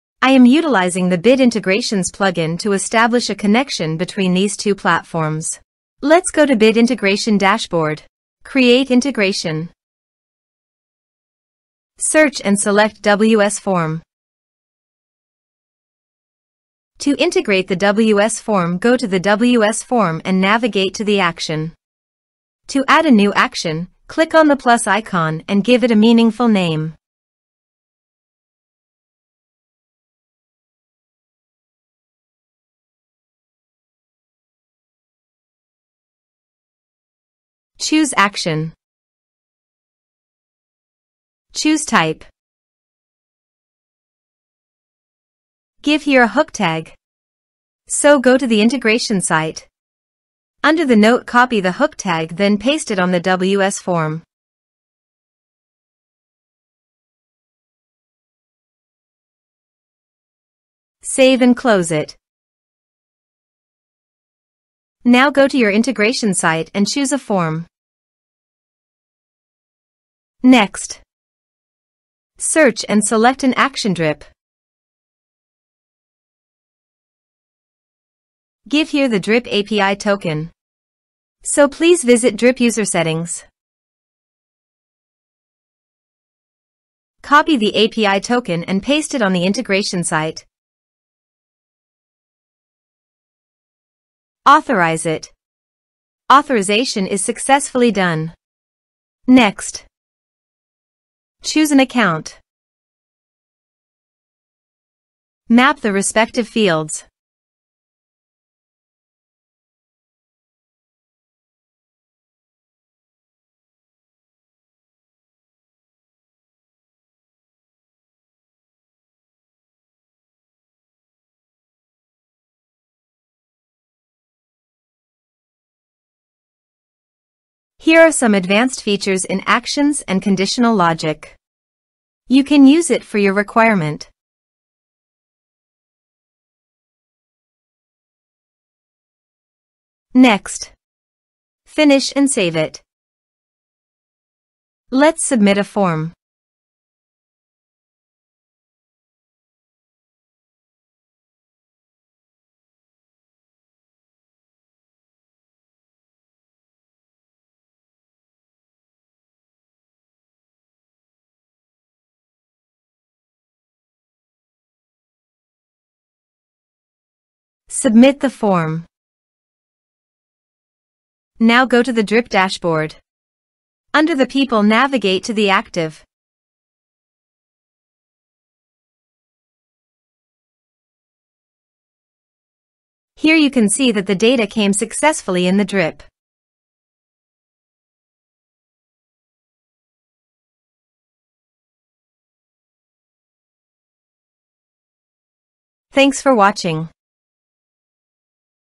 Integrating WS Form with Drip | Step-by-Step Tutorial | Bit Integrations
🔗 Ready to supercharge your email marketing with seamless form integration? Join us on our YouTube channel for an in-depth tutorial where we demonstrate the integration of WS Form with Drip using the Bit Integrations WordPress plugin. Learn how to effortlessly connect these tools to automate your email campaigns and manage your subscribers effectively.

Follow along as we guide you through the step-by-step process of integrating WS Form with Drip, enabling you to automate your email workflows, enhance customer engagement, and boost productivity. Say goodbye to manual email tasks and welcome automated workflows for increased efficiency.

Key Highlights:
✅ In-depth walkthrough for integrating WS Form with Drip using Bit Integrations for WordPress.
✅ Leverage your form data to automate email marketing tasks and manage subscribers with Drip.
✅ Elevate your marketing strategy with streamlined automation and increased productivity.

Ready to revolutionize your email marketing workflow?

Unlock advanced email marketing capabilities:
📧 Elevate your automation endeavors with the Pro version of Bit Integrations

Whether you're a marketer, business owner, or WordPress enthusiast, this integration empowers you to seamlessly merge WS Formwith Drip. Watch now to revolutionize your email marketing efforts!

🎥 Ready to revolutionize your email marketing workflow? Join us in optimizing your marketing strategy with seamless integration! 👍